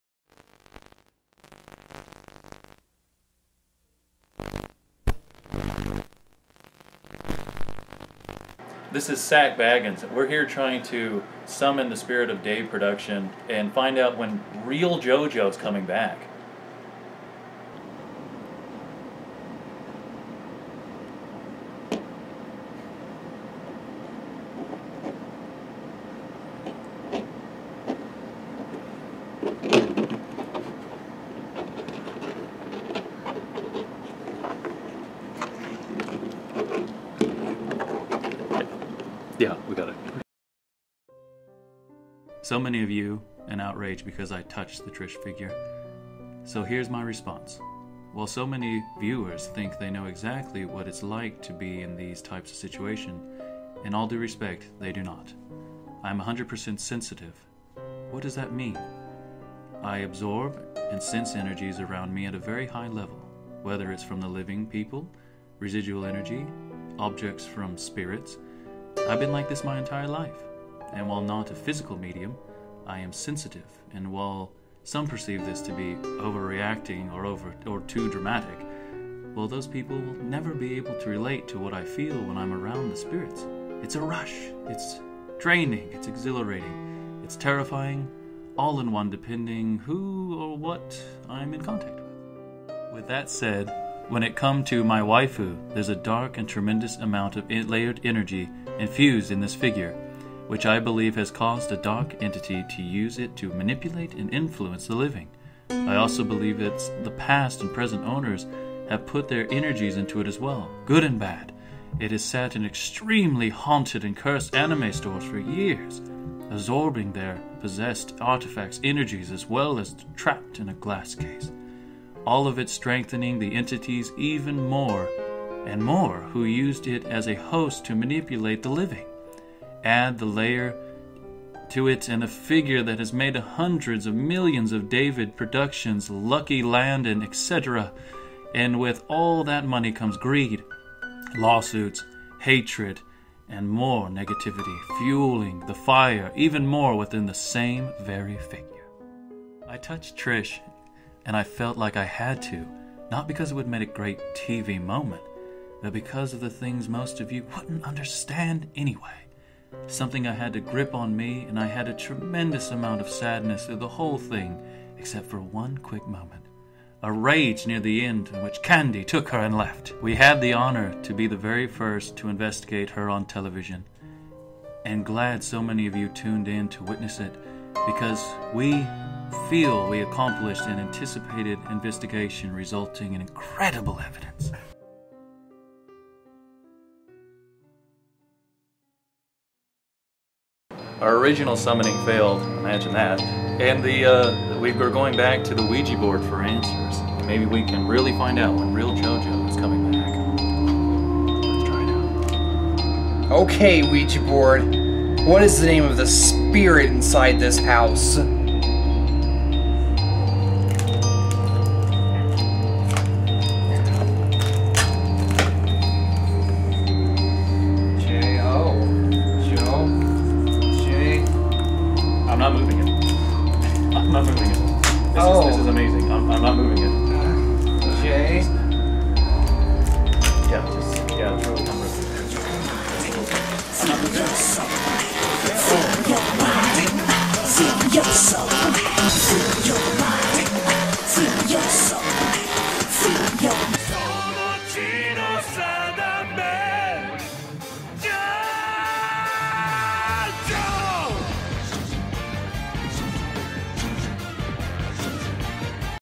[5/13] Bambino Crusaders: Bento Oreo and Touching The Trish Figure
In this lost episode of Skost Inventor, the investigation into the haunting of Dave Production continues after a controversial first airing.